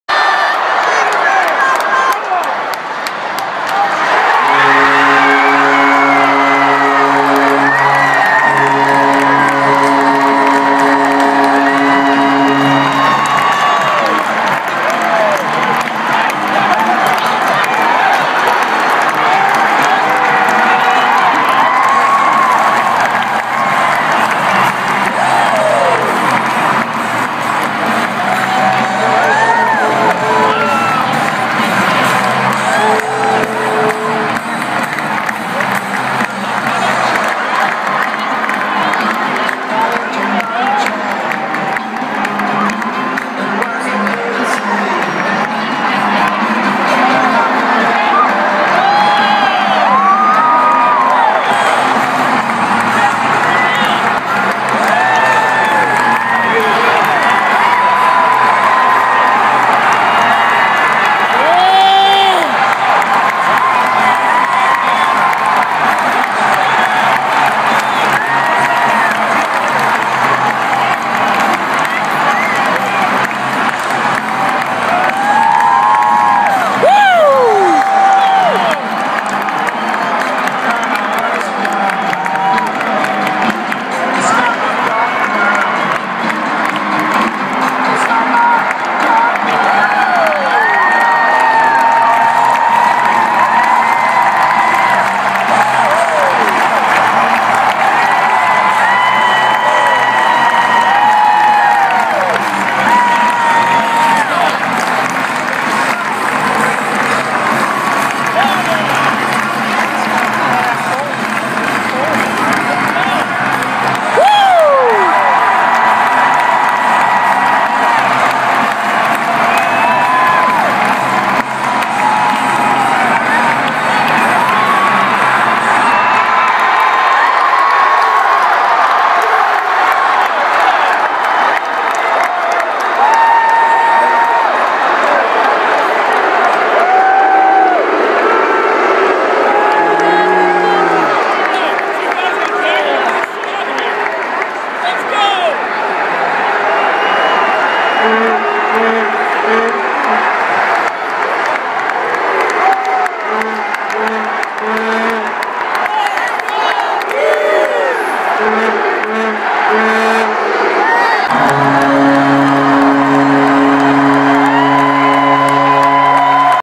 Blues vs. Jets 3/17/14 Goal 4 + Win & Crazy Brawl!
Absolute chaos after the final buzzer! Must watch!